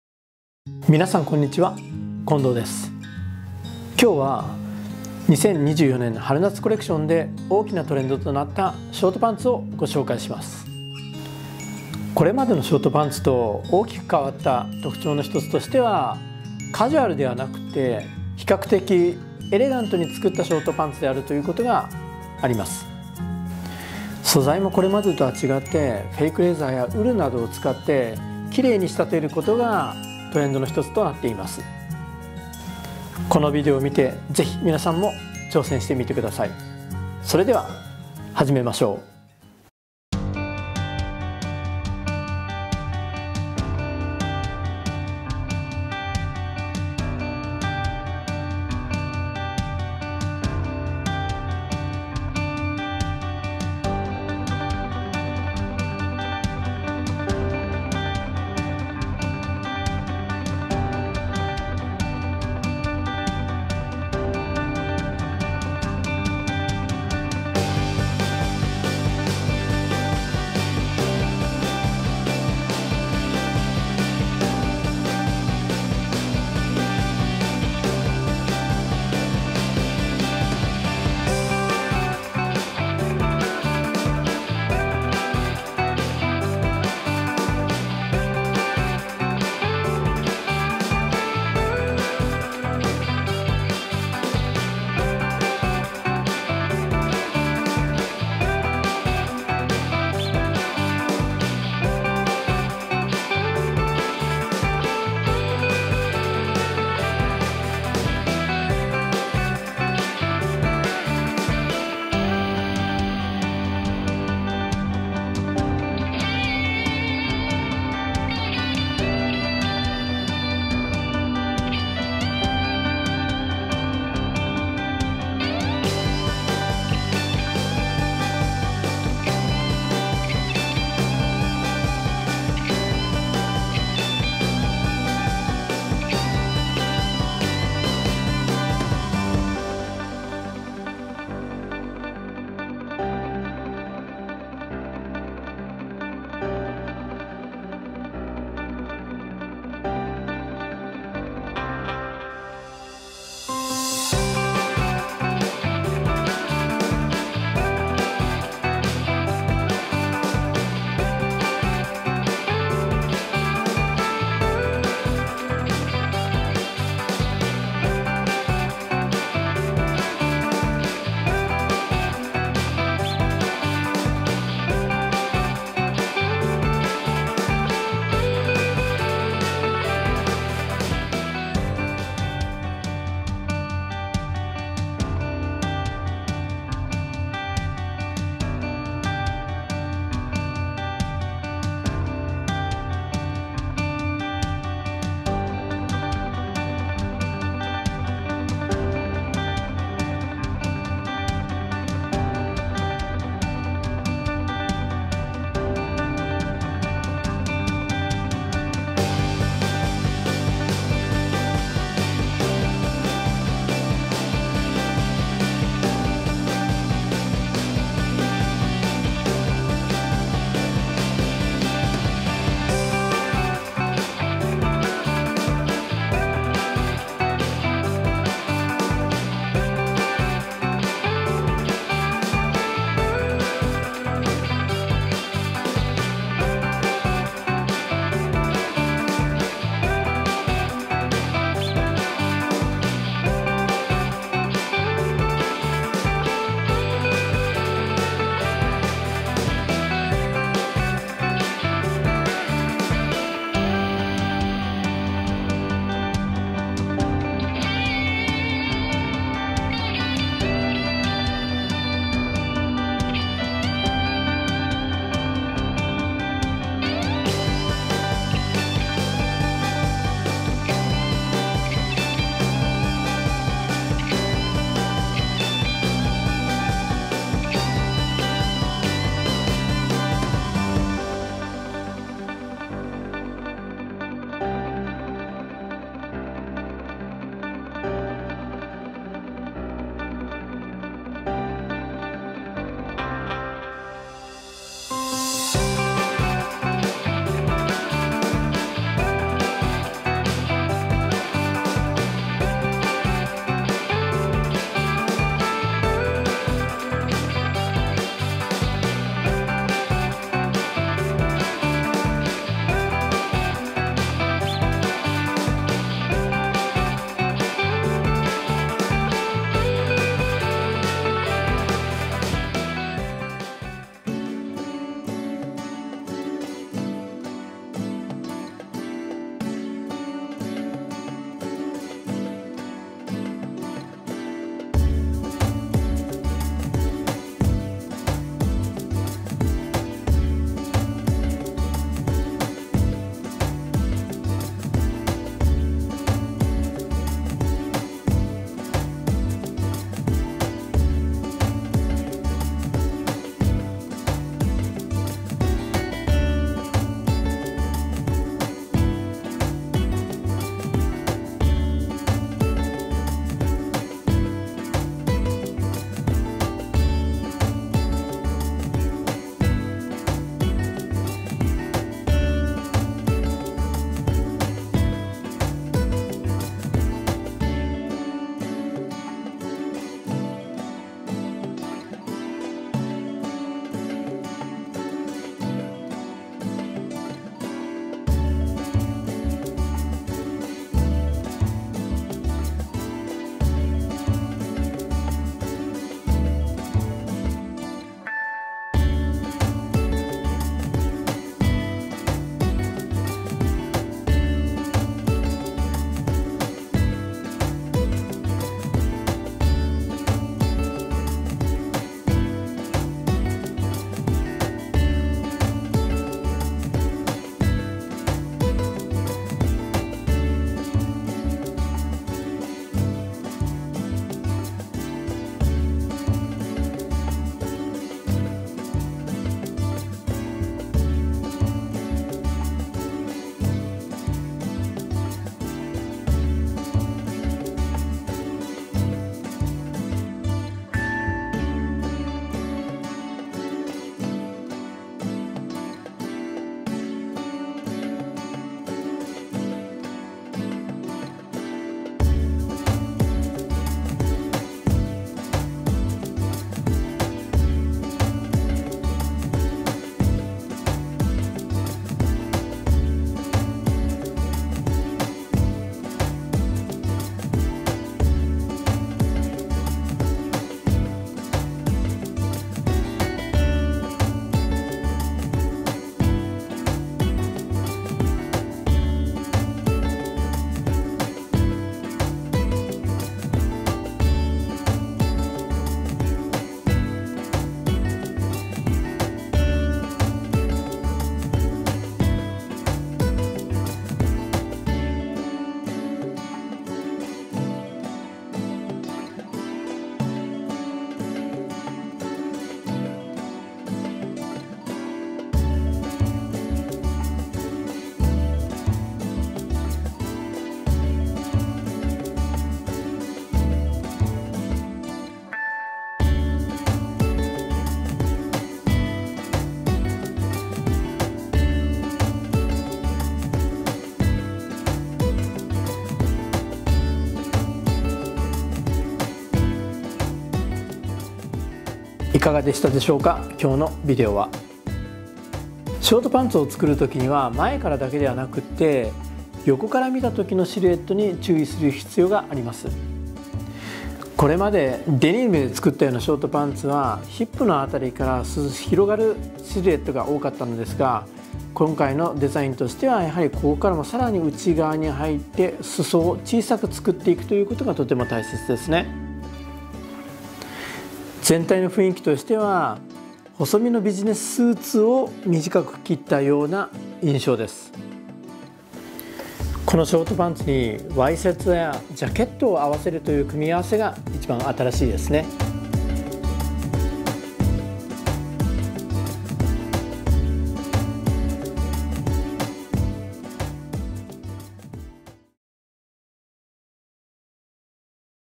【Shorts】The Library of Draping, Kondo's draping archive, ショートパンツ
This video is about the draping of narrow shorts made into elegant silhouettes. The trend for casual, rough trousers seems to be over.　今回のビデオはエレガントなシルエットに作られた細めのショートパンツのドレーピングです。典型的なカジュアルでラフな雰囲気のショートパンツの流行は終わりですね。

Which dress form I used:
SPUR SPU-PA-MC36 (Global model size 36)
Scissors: JS-DA-R26
Ruler: 50cm SW50
Curve ruler: KL
Masking tape